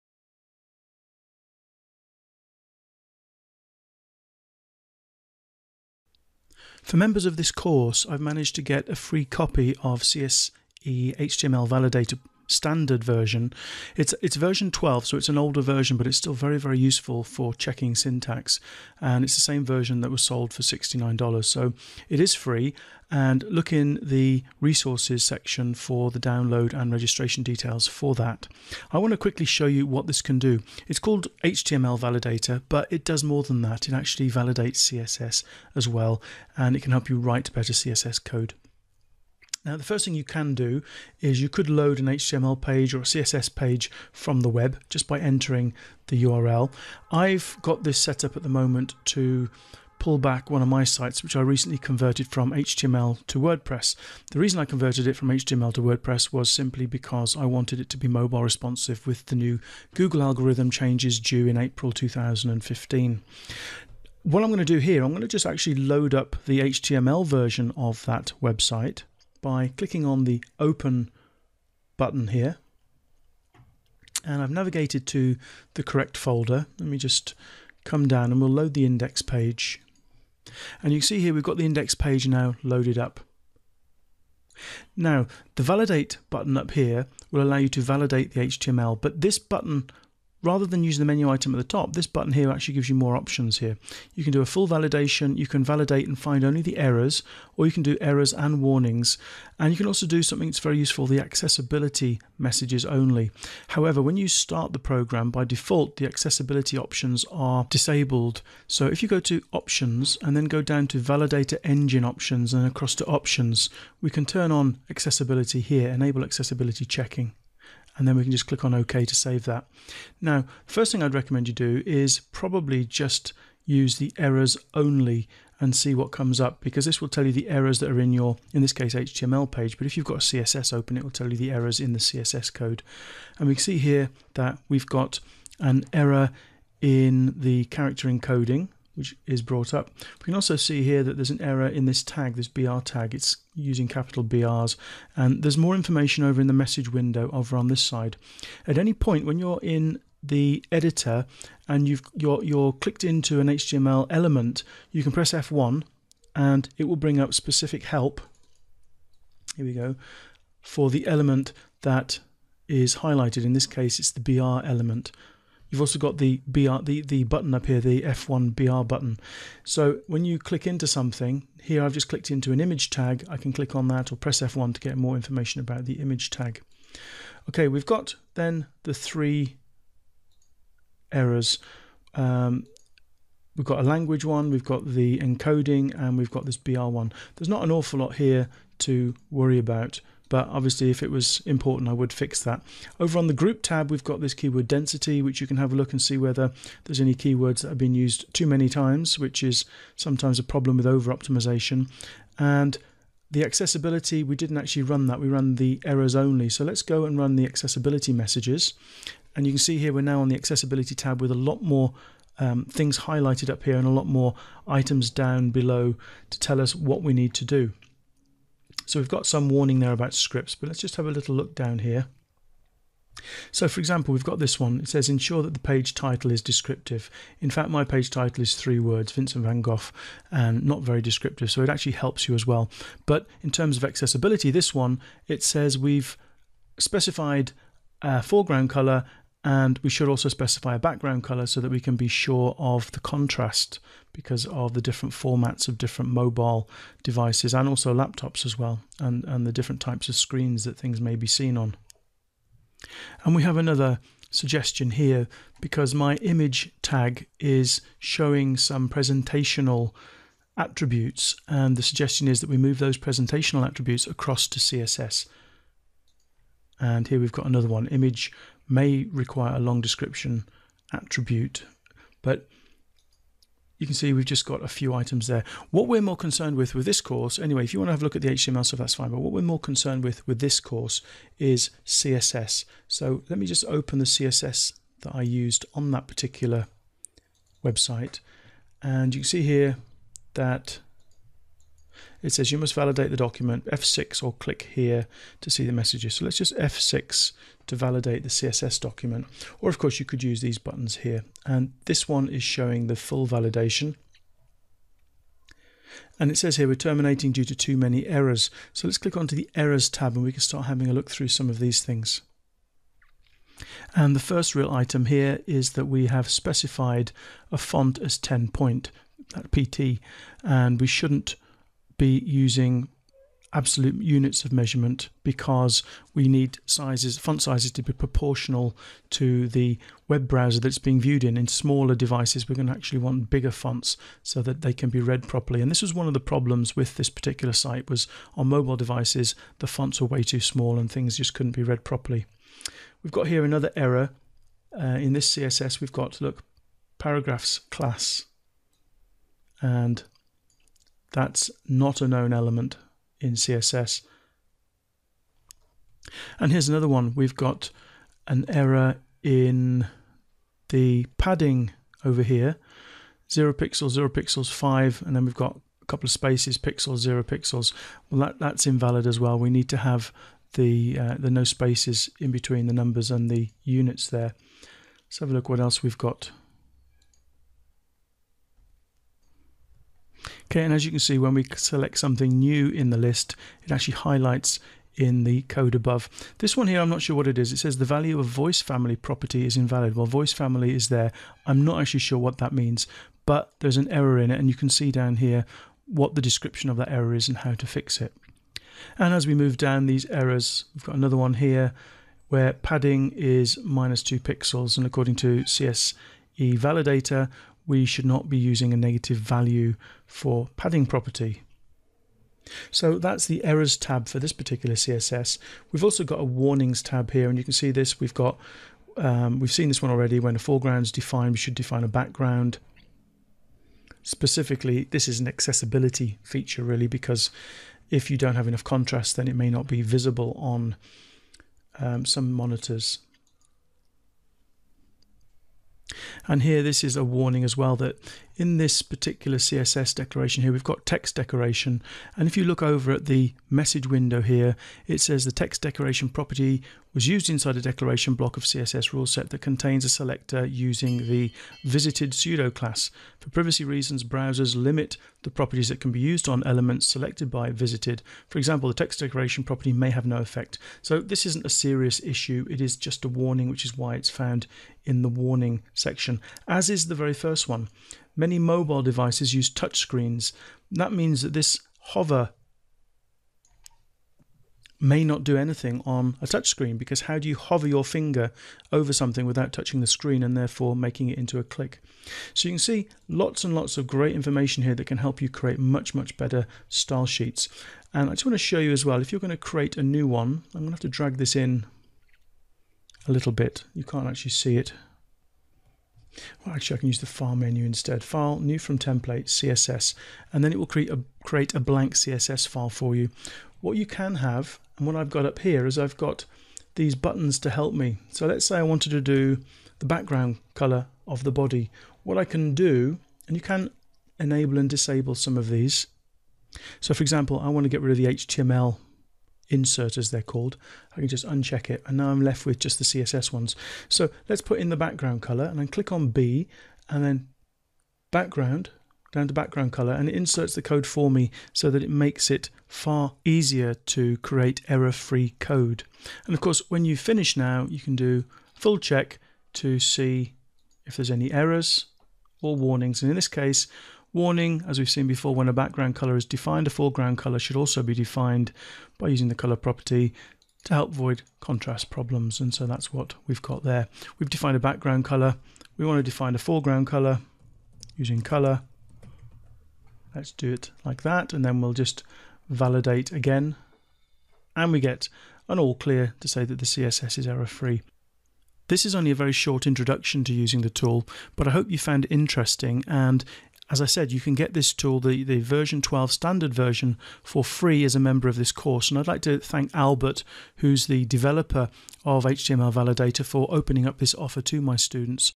CSE HTML Validator
CSE HTML Validator is much more than an HTML validator.  I am using it to validate my CSS, and it's working great.  This quick demo video shows the main features of CSE Validator that I use.
You can get CSE HTML Validator 12 standard for free when you sign up for my CSS course here: